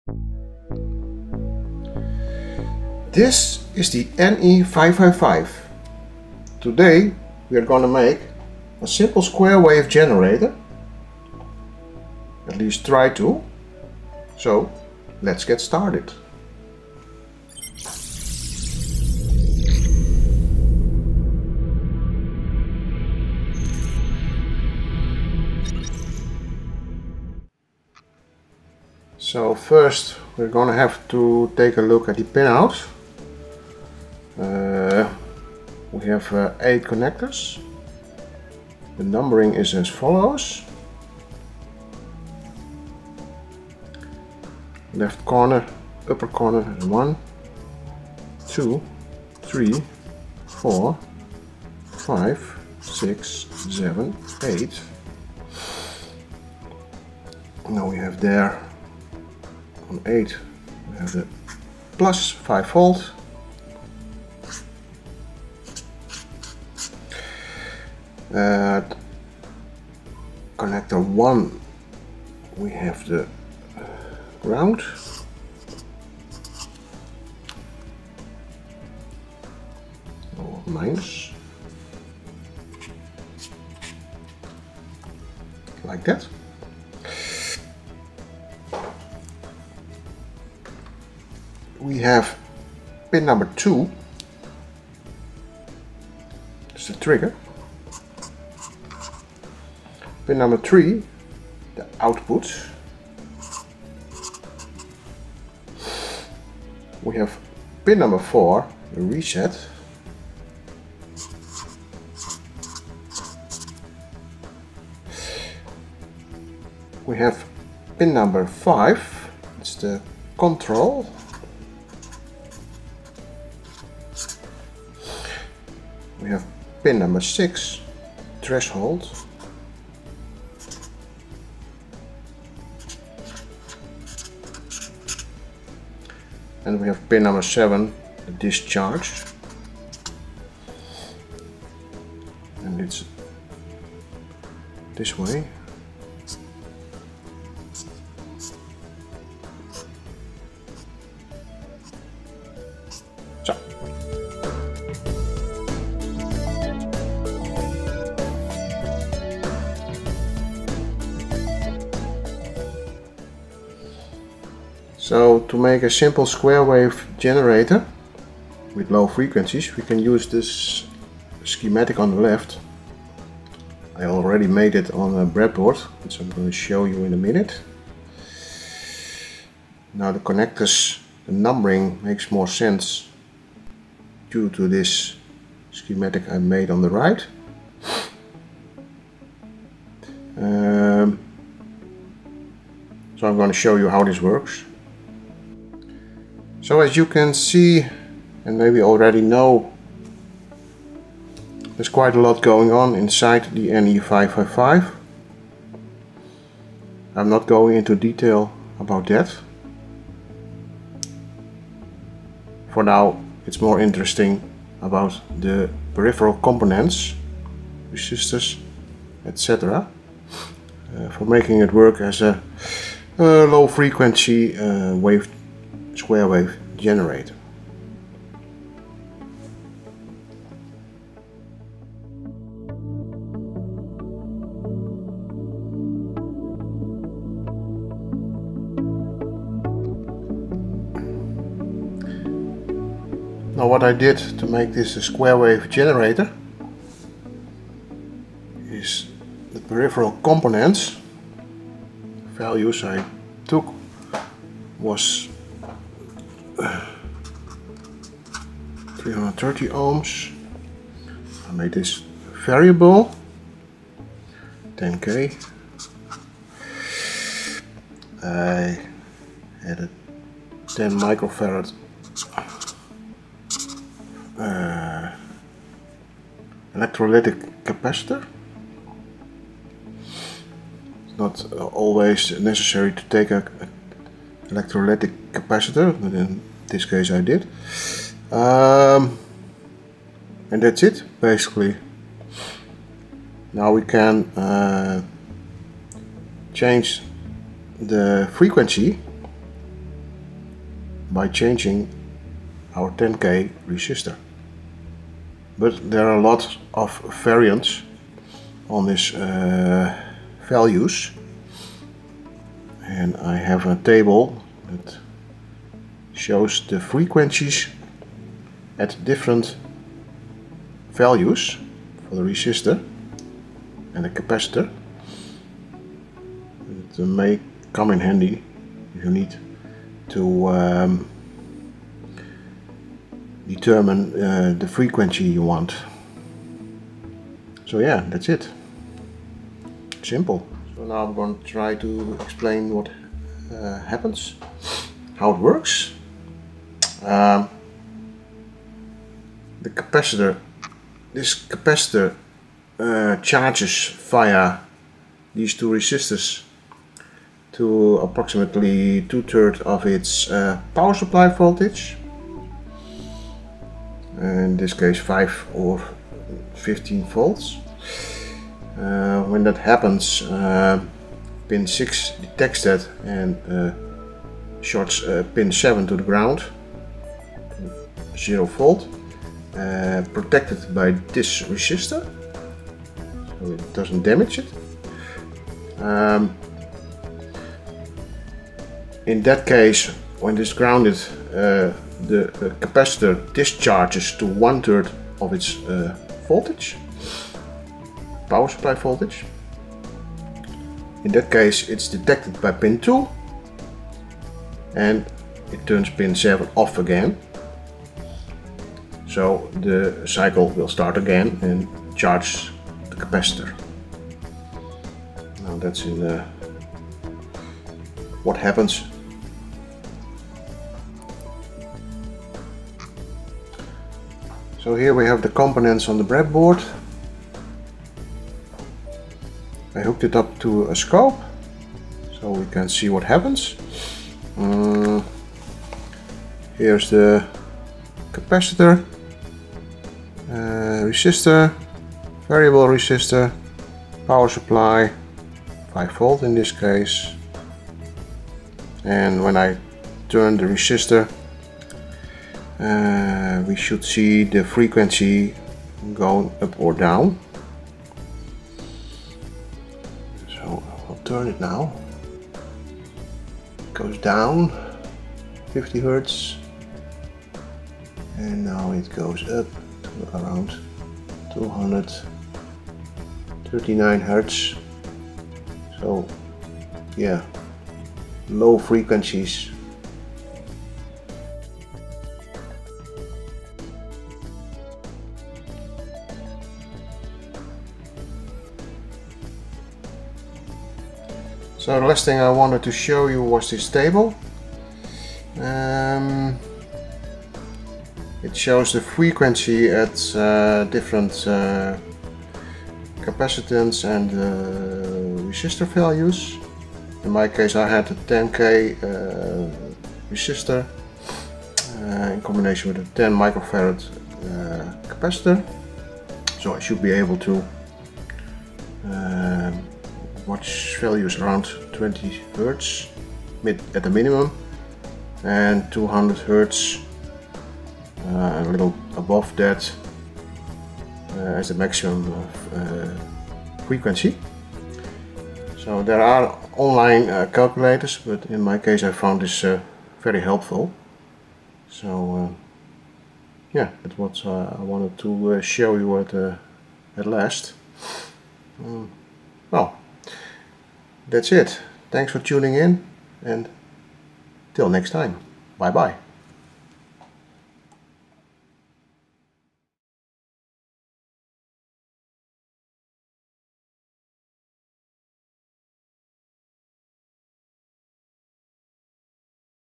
NE555 SIMPLEST square wave generator
NE555 simplest square wave generator (astable).
Here we try to make a square wave generator with the NE555 ic.
It is possibly the most-frequent used chip and easy to use.